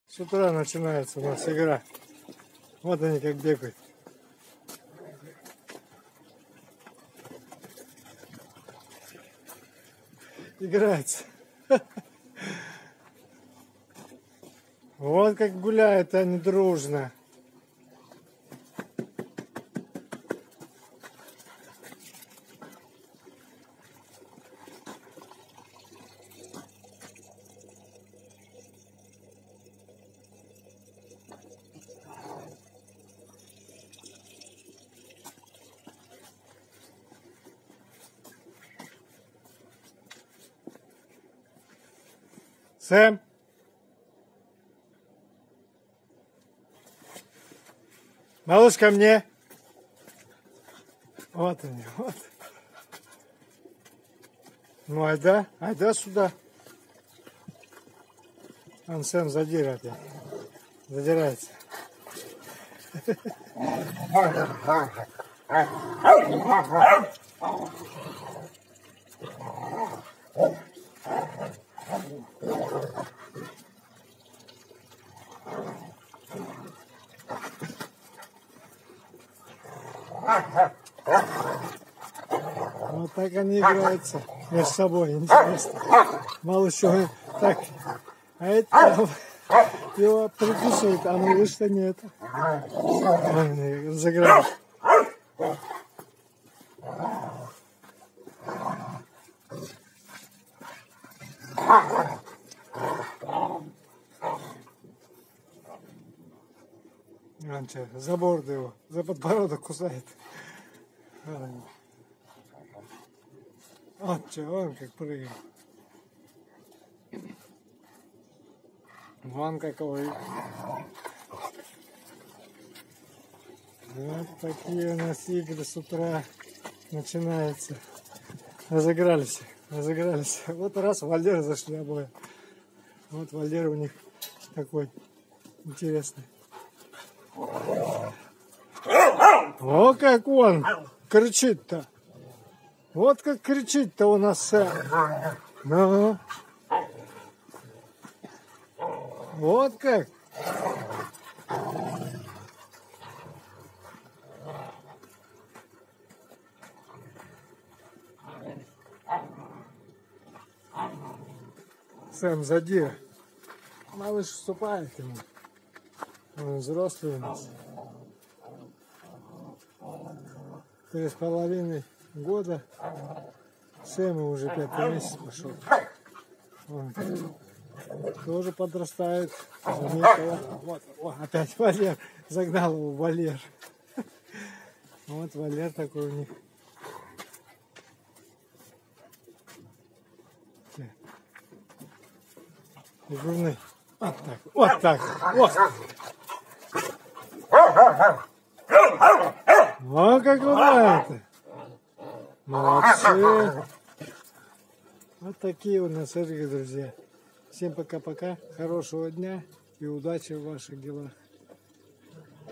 Ротвейлер Малыш и Сэм,утренние игры и разминка
Малыш и Сэм игры разминка с утра,советую посмотреть всем